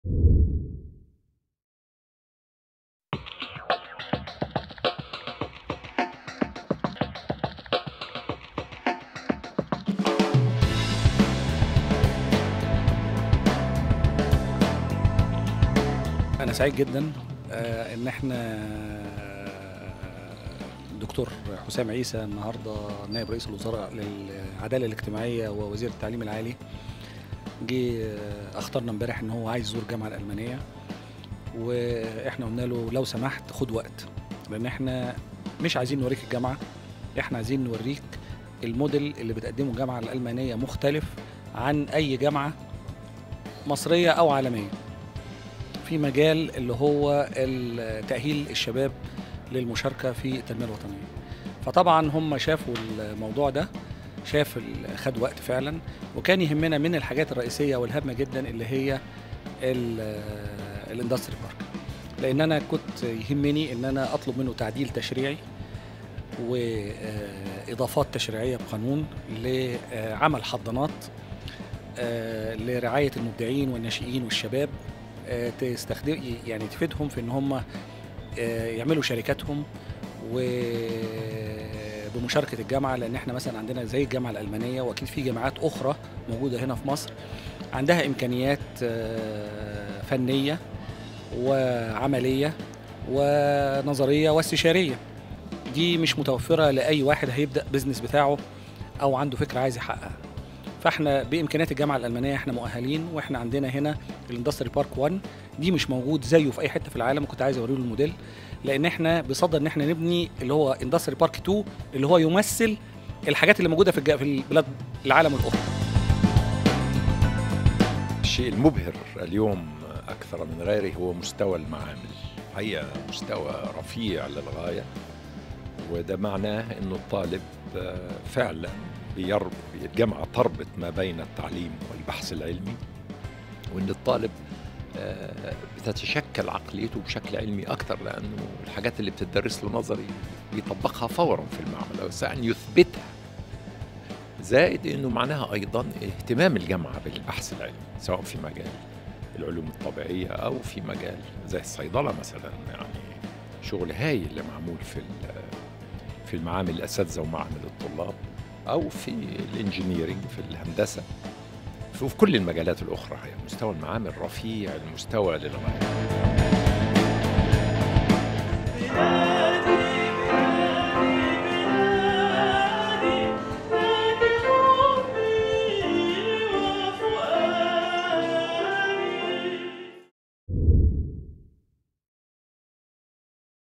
Day by Day 4-2-2014
GUC hosted the Deputy Prime Minister and Minister of Higher Education Hossam Eissa in a one-day-tour at its campus. Besides, Dr. Eissa paid MPC's studios and offices a visit during the tour.